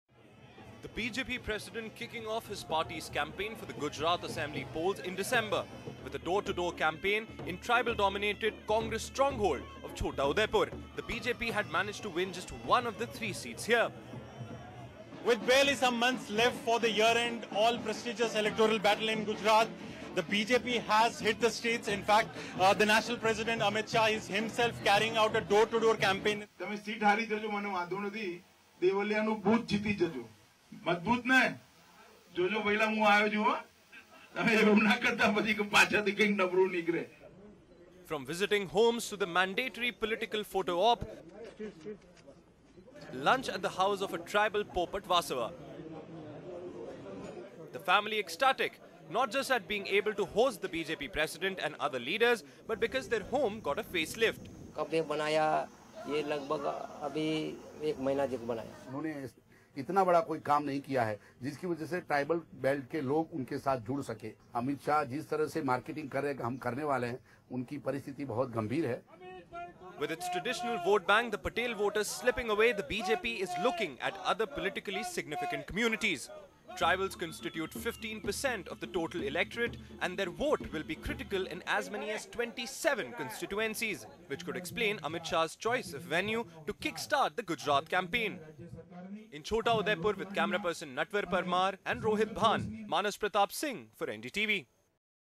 In Congress Stronghold, Amit Shah Sets 150-Seat Target For BJP In Gujarat
Workers of the BJP will fan out in 48,000 booths to reach the maximum number of people before assembly elections Gujarat, as the party seeks to retain the state for the sixth time.  The target is to win 150 of Gujarat's 182 seats, said BJP chief Amit Shah on Wednesday, offering a simple formula. "We won 120 seats when Narendrabhai was Chief Minister. Now when he is Prime Minister, we should win 150," Mr Shah said.